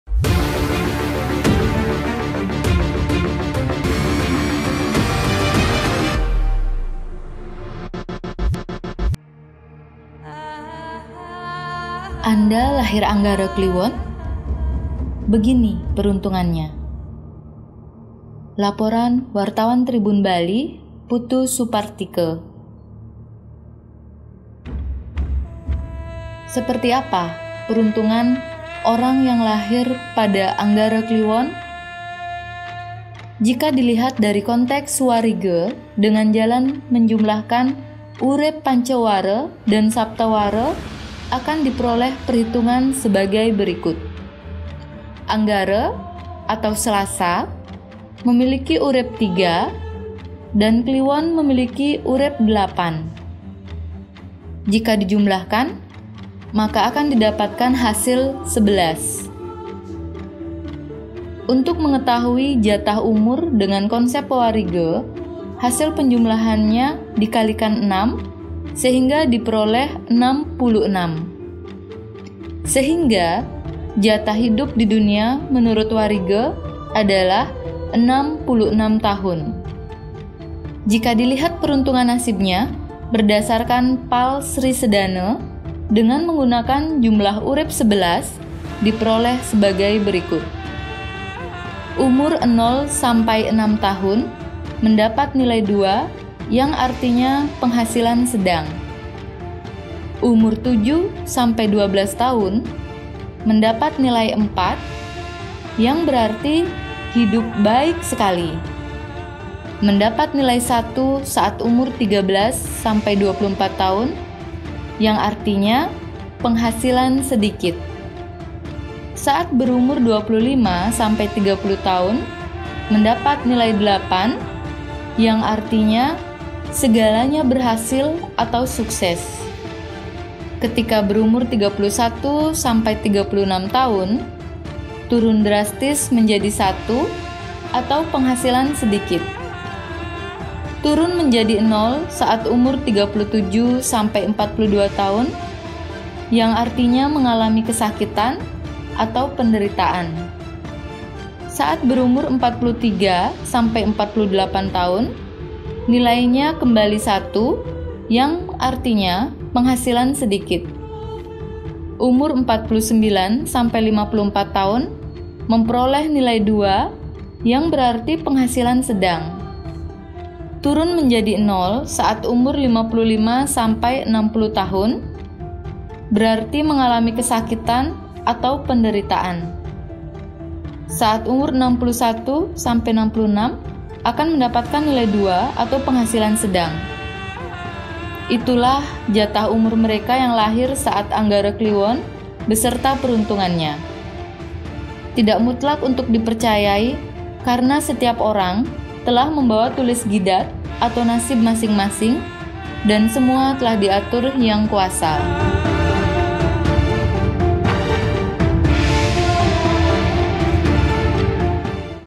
Lahir Anggara Kliwon? Begini peruntungannya
Anda lahir anggara kliwon Dukut?

Hari ini, Selasa (2/10/2018) merupakan otonannya.

Jika dilihat dari konteks wariga dengan jalan menjumlahkan urip pancawara dan saptawaranya akan diperoleh perhitungan sebagai berikut.

Anggara (Selasa) memiliki urip 3 dan Kliwon memiliki urip 8.

Jika dijumlahkan maka akan didapatkan hasil 11.

Untuk mengetahui jatah umur dengan konsep wariga, hasil penjumlahannya dikalikan 6 sehingga diperoleh 66.

Sehingga jatah hidup di dunia menurut wariga adalah 66 tahun.